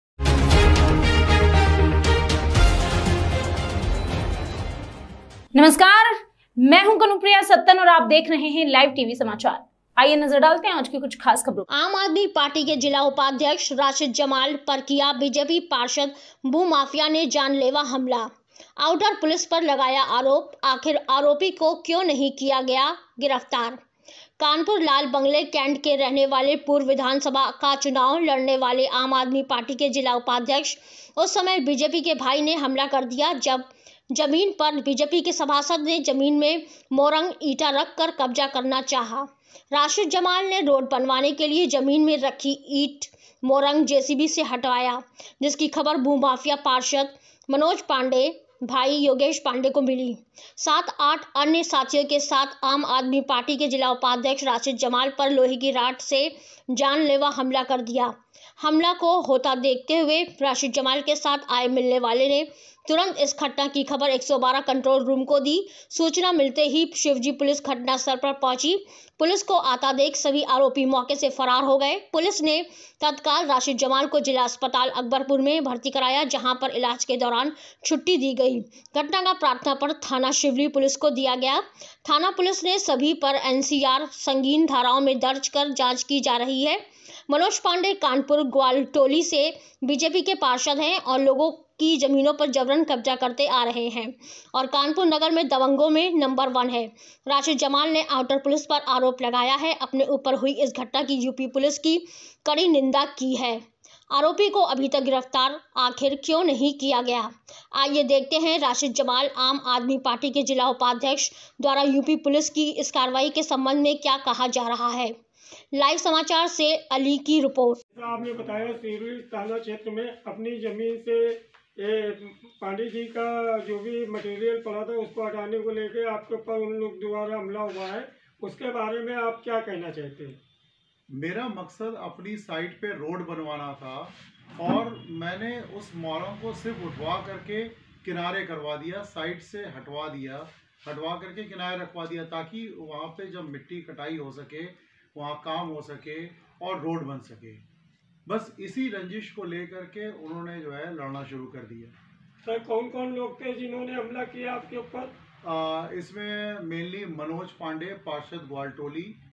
कानपुर : आम आदमी पार्टी के जिला उपाध्यक्ष राशिद जमाल पर किया बीजेपी पार्षद भूमाफिया ने जानलेवा हमला*
*आम आदमी पार्टी के जिला उपाध्यक्ष राशिद जमाल पर किया बीजेपी पार्षद भूमाफिया ने जानलेवा हमला*